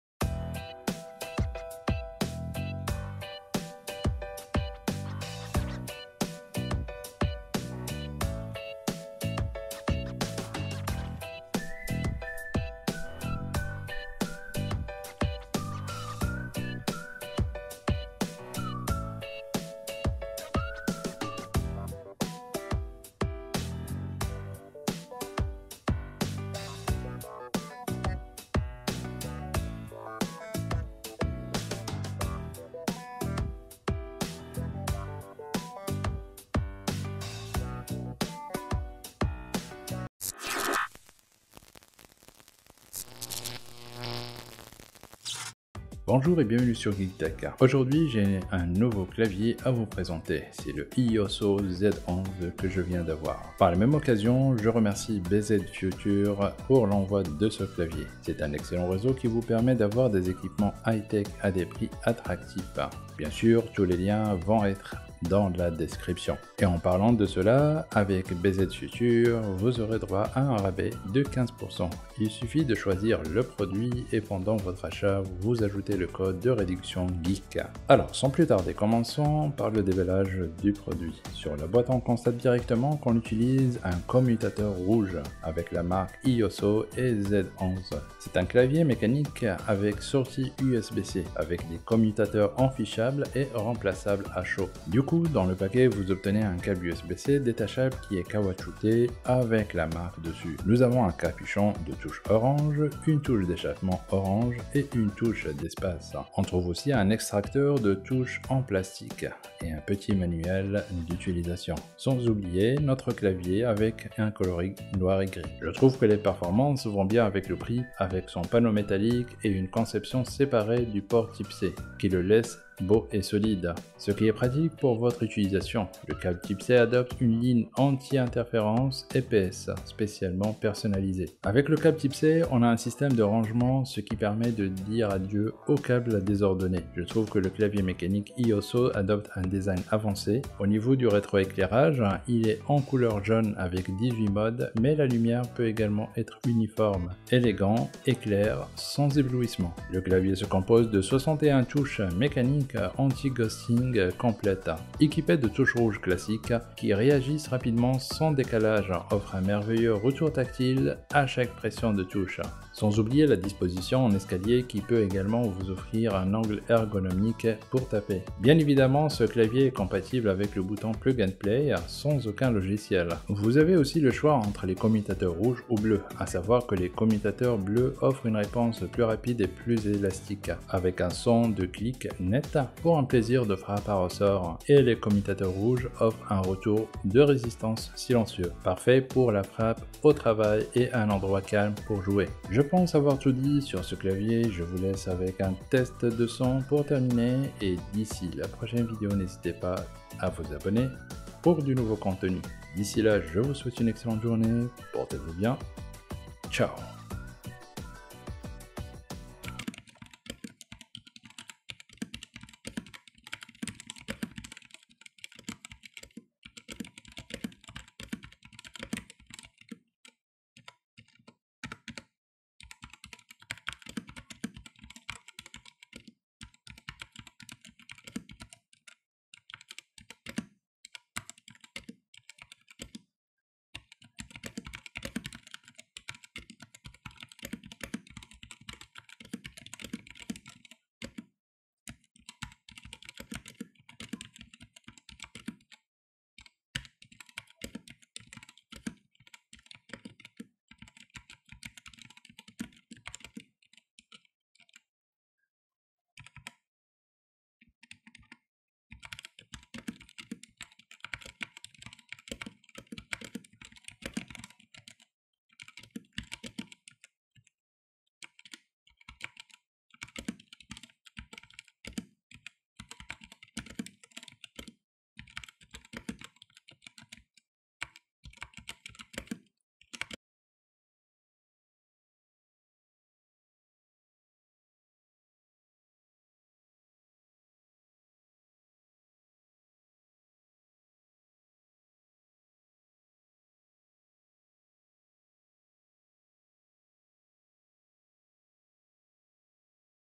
E-Yooso Z11 - Un Clavier de JEU à 61 Touches ! Par BZFUTURE
Voici un autre excellent clavier de jeu RVB complet compact par E-Yooso fourni par BZFUTURE.COM, le Z11 est un clavier très économique qui offre beaucoup de fonctionnalités pour le prix et je l'aime vraiment bien !